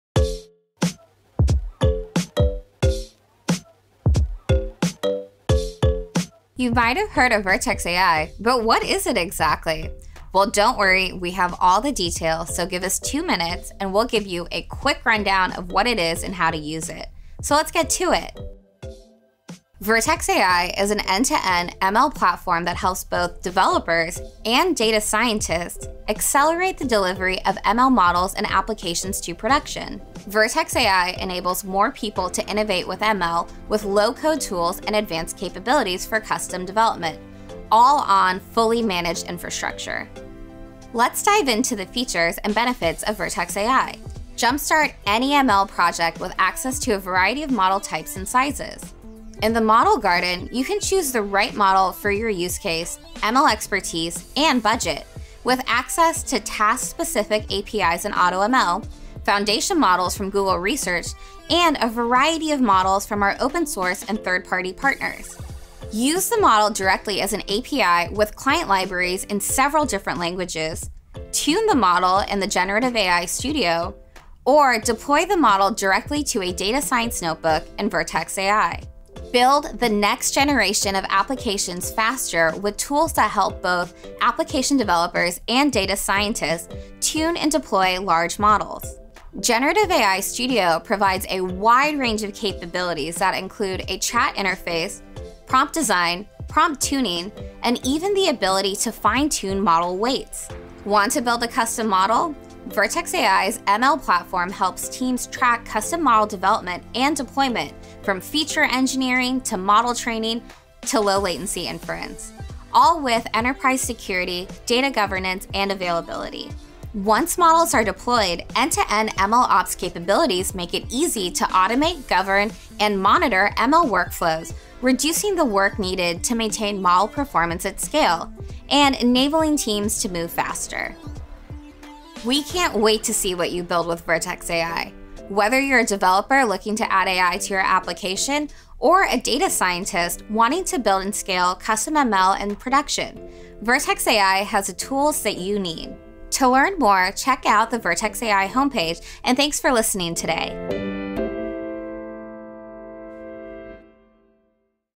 Introduction to Machine Learning on Vertex AI
In this first video, we discuss Generative AI on Vertex AI. Watch along and learn about low-code tools and advanced capabilities for custom AI and ML development.

Chapters:
0:00 - Intro
0:19 - What is Vertex AI?
0:43 - What is Model Garden?
1:18 - What is Generative AI Studio?
1:40 - Custom Modeling on Vertex AI